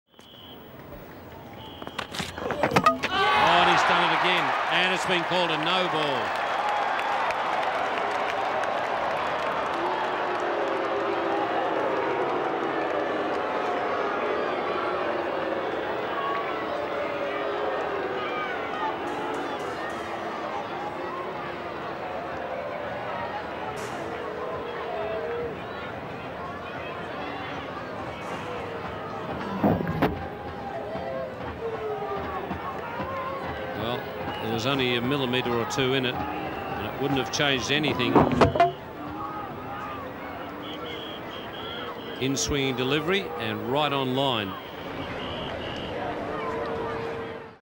Brett Lee sends stumps flying but its a no ball! | From the Vault
Oh no Binga! Brett Lee takes Damien Wright's middle peg out of the ground in the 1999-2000 Mercantile Mutual Cup, but the Aussie quick didn't keep his front foot behind the line!